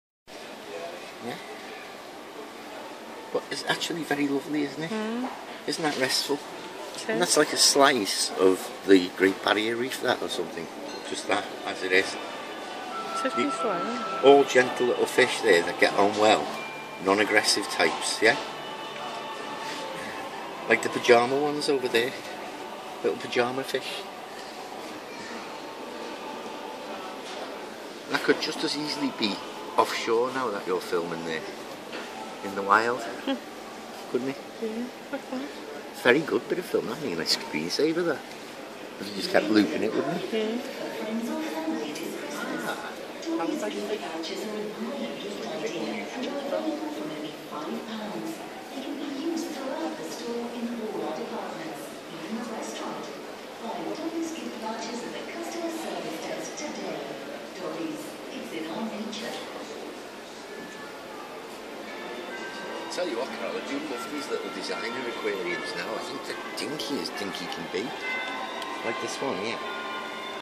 Living coral, elephant ear and toad stool corals
Corals are marine animals in class Anthozoa of phylum Cnidaria typically living in compact colonies of many identical individual "polyps". The group includes the important reef builders that inhabit t